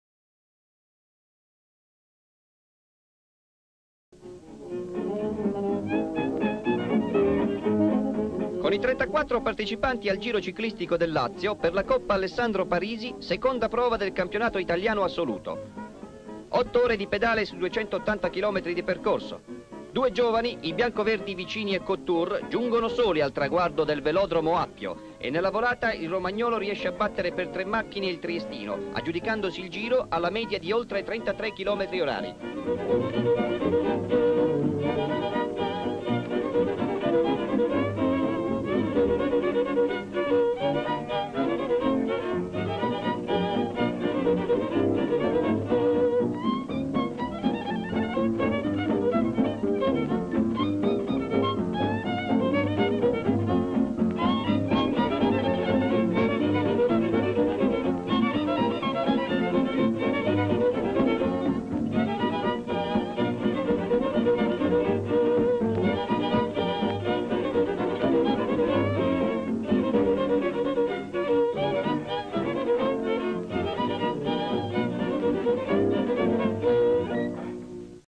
Italia Roma
Descrizione sequenze:alcuni ciclisti tra cui Bartali ; il gruppo di ciclisti parte per il percorso che è di 280 km ; fasi della gara ; i due ciclisti Vicini e Cottur arrivano in solitaria al velodromo Appio ; il romagnolo Vicini batte in volata il triestino Cottur ;